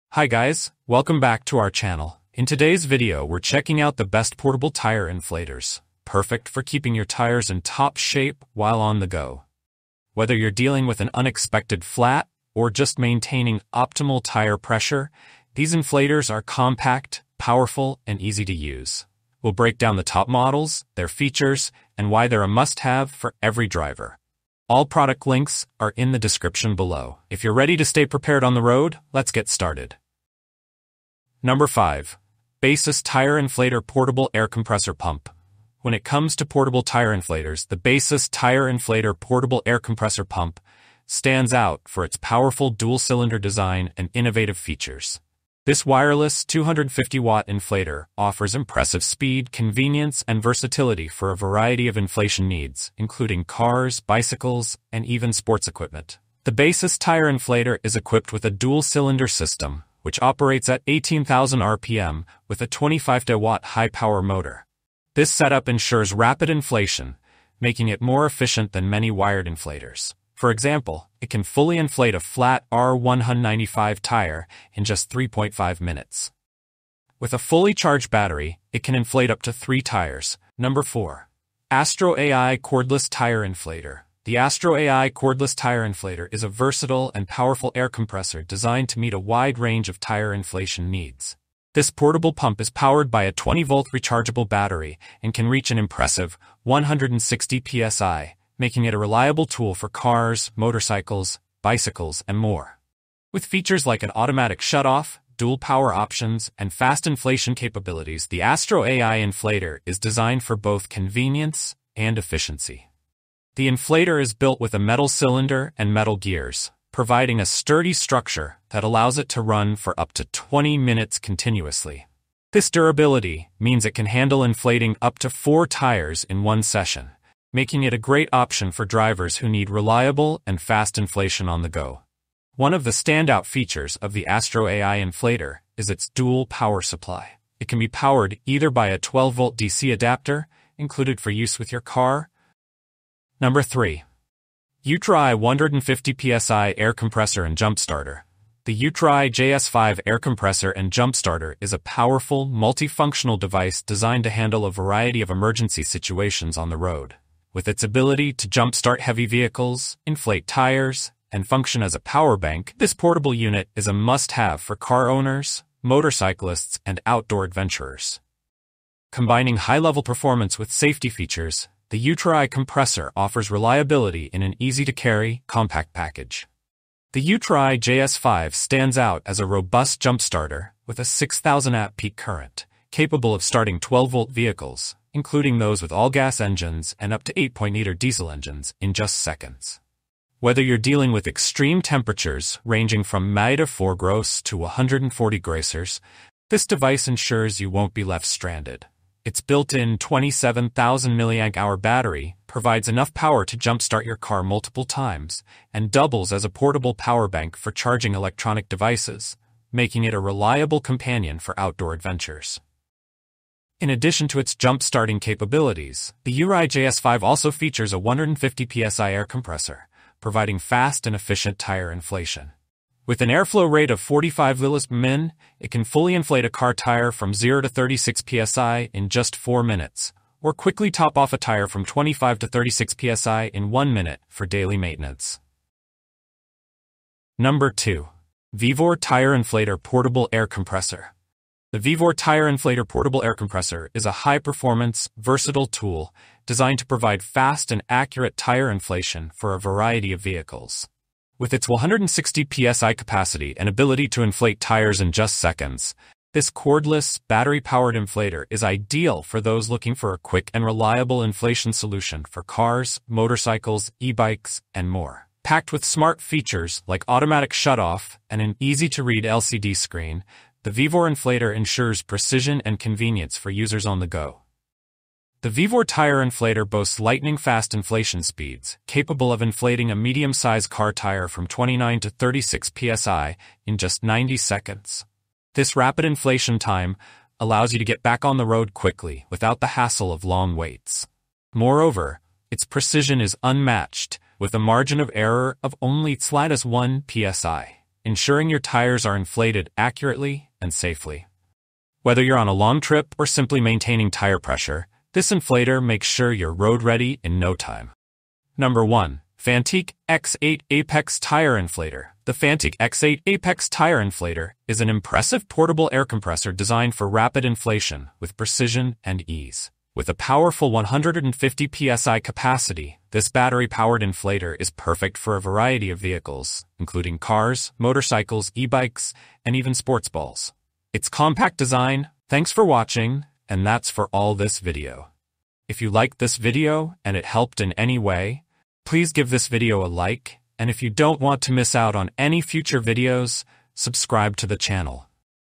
Top 5 Best Portable Tire Inflators 2025 - SmartBest!
The Products Shown in The Video:
5. Baseus 250W Portable Tire Inflator ✅
4. AstroAI Portable Air Compressor ✅
3. UTRAI Compressor 150PSI Air Pump ✅
2. VEVOR Tire Inflator Portable Air Compressor ✅
1. Fanttik X8 APEX Tire Inflator ✅